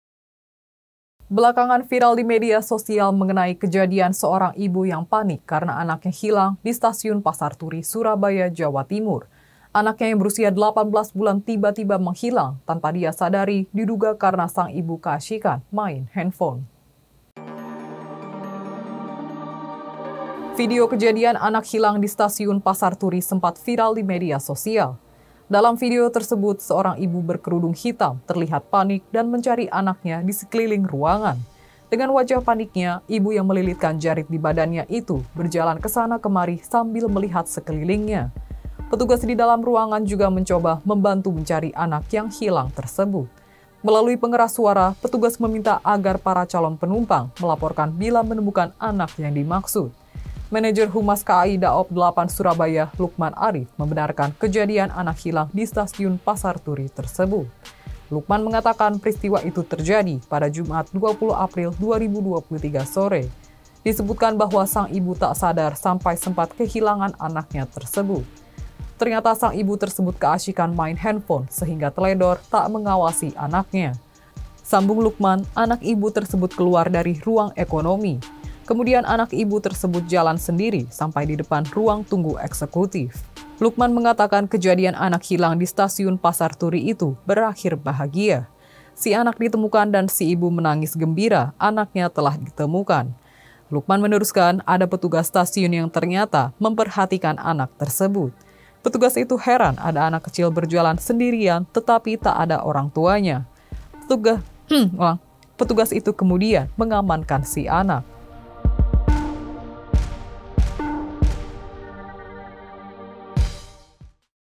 Viral Ibu Panik Cari Anak Hilang Teledor Asyik Main HP
Anaknya yang berusia 18 bulan tiba-tiba menghilang tanpa dia sadari diduga karena sang ibu keasyikan main handphone (HP).